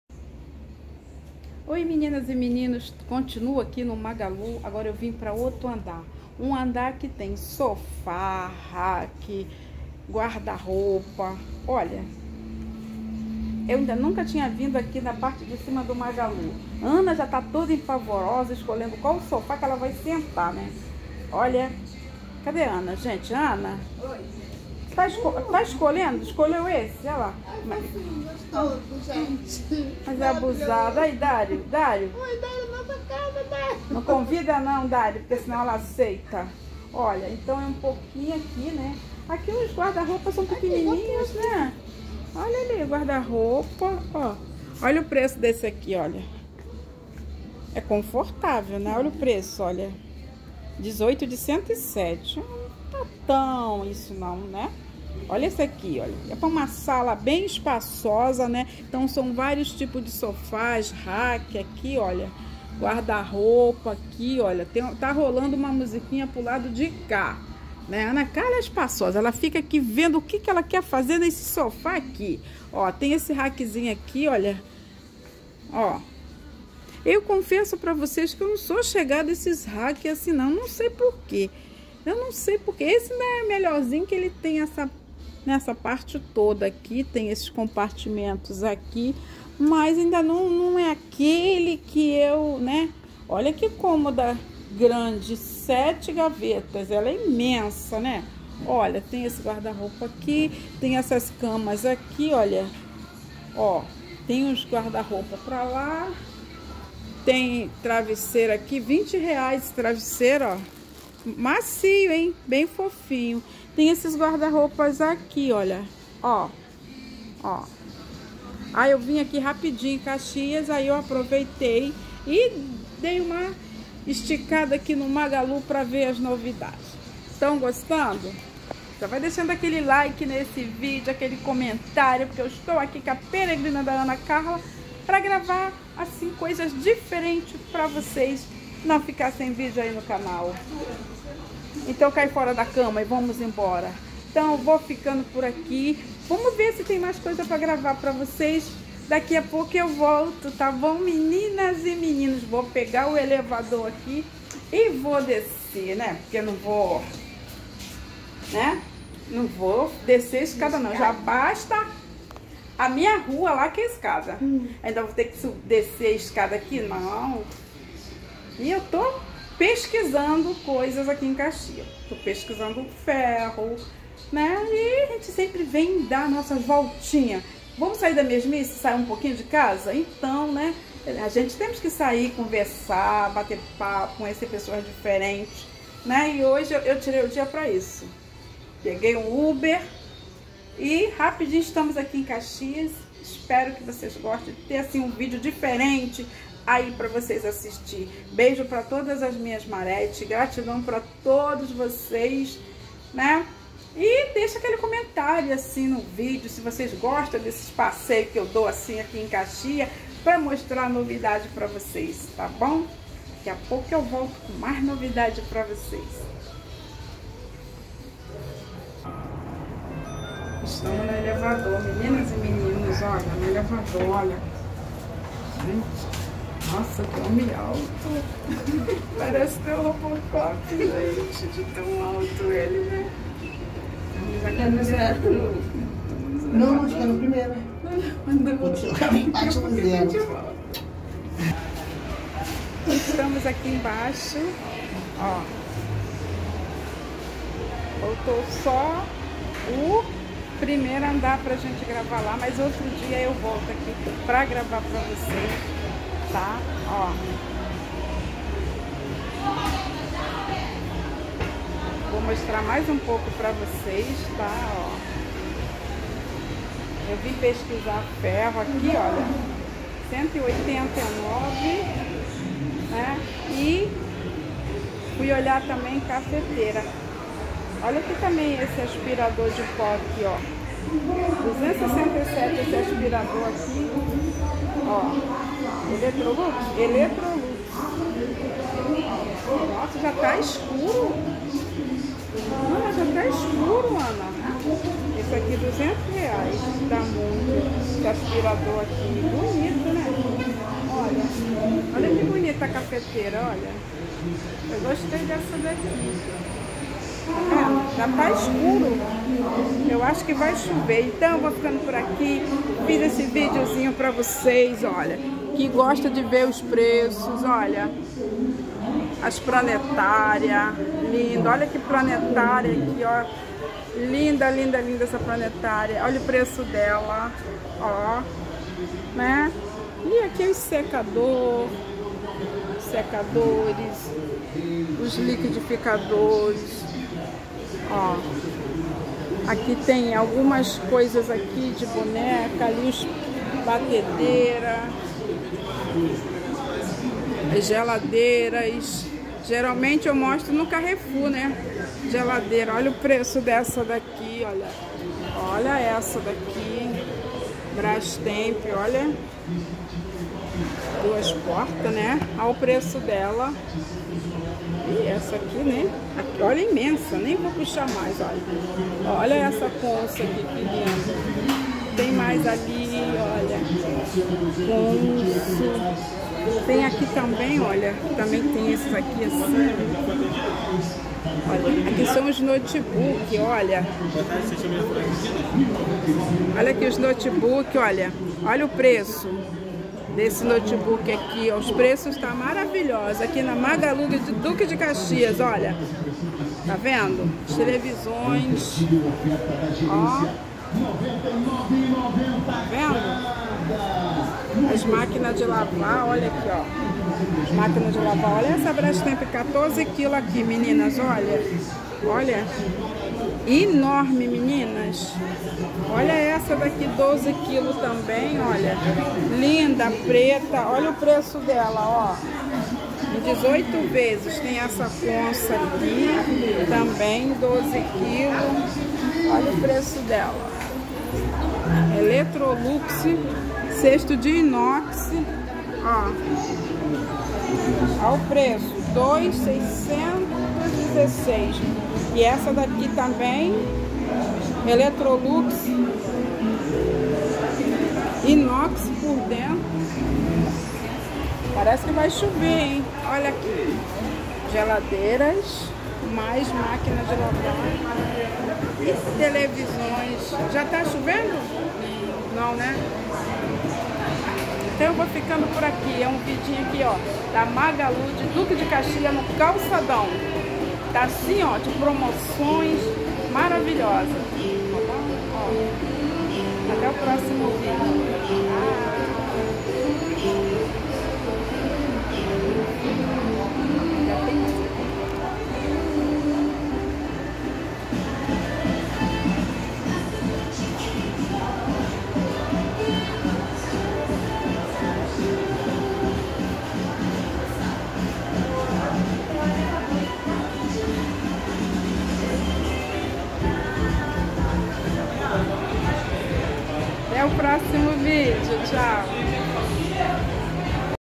ainda estou no magazine Luiza pesquisando ferro e cafeteira 💞😍
boa tarde pra todos vocês gratidão sempre 💋❤️.@canal do Juju. canal Dario Moraes